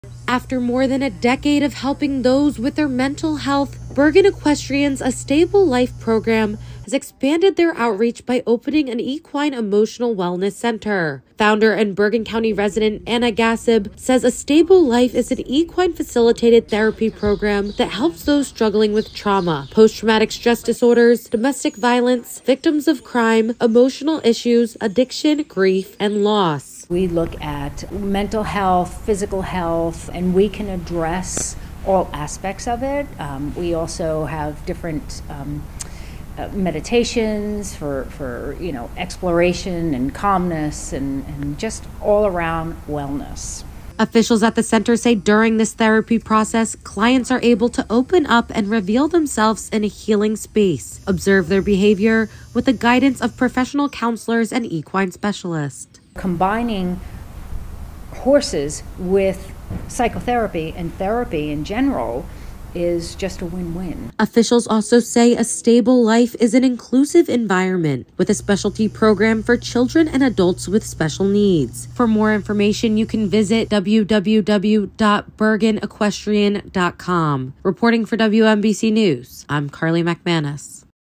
Bergen Equine Center First Equine Wellness Center in N.J.
Bergen Equestrian Center (BEC) opens Bergen County's first equine emotional wellness center, A Stable Life, located in Overpeck Park in Leonia, New Jersey, about 10 minutes from the George Washington Bridge. A Stable Life is an equine-facilitated therapy program founded by Bergen County resident Anna Gassib, who is an Associate Counselor in Equine therapy, Trauma, Gestalt, and Person-Centered therapies.  Anna and her team of professionals, offer customized unique workshops, designed by a caring and skilled group of mental health experts, to help those struggling with trauma, post-traumatic stress disorders, domestic violence, victims of crime, emotional or relationship issues, addiction, grief, loss, and more.

A Stable Life is a well-respected wellness center, focusing on mental health and emotional well-being, working with individuals and agencies such as Women's Rights Information Center, Bergen Family Center, Bergen County Spring House- a halfway house for those recovering from addictions, Covenant House of Newark.

Utilizing its interdisciplinary approach to healing, coupled with the exceptional qualifications of its staff, A Stable Life offers a variety of services for those looking for healing.  Cultural competency is a top value and treatment team members are comprised of licensed professionals who are representative of the community it serves, including a diversity of religions, cultures, and those who identify as LGBT+.